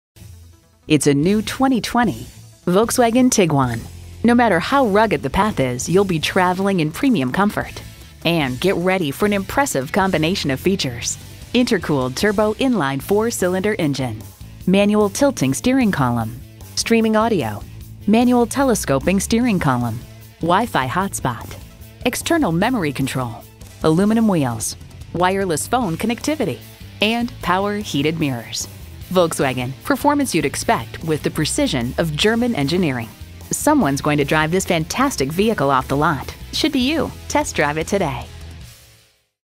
2020 Volkswagen Tiguan Baltimore MD Parkville, MD O0027579 - SOLD
Year: 2020
Make: Volkswagen
Model: Tiguan
Trim: S
Engine: 2.0L TSI DOHC
Transmission: 8-Speed Automatic with Tiptronic
Color: Silver
Interior: Titan Black
Stock #: O0027579
VIN: 3VV1B7AX8LM027579

Address:
9215 Harford Rd